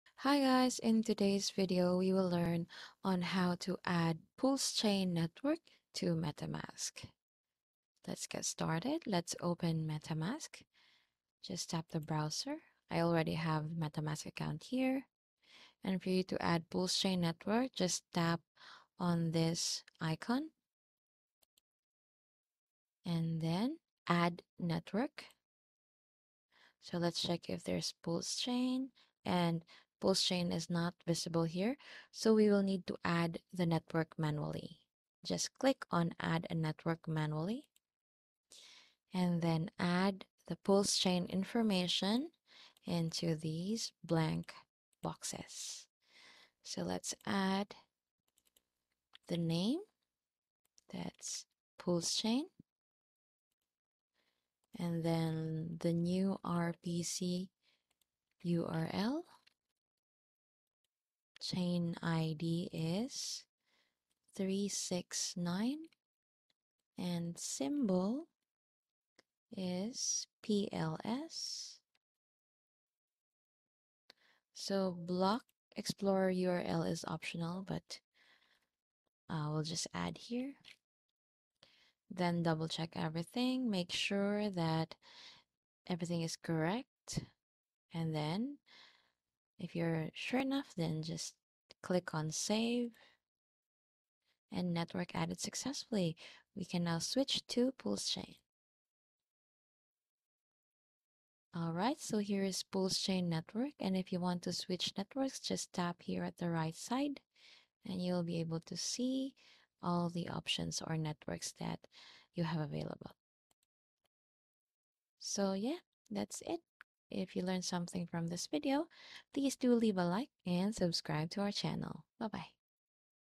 How to add Pulsechain network to Metamask (2024)!! | FULL GUIDE
In this quick tutorial, we'll show you how to link your Pulsechain Network to your Metamask Wallet! If you find other tutorials confusing, what better way than to follow a easy guide on how to do that. Safe investing!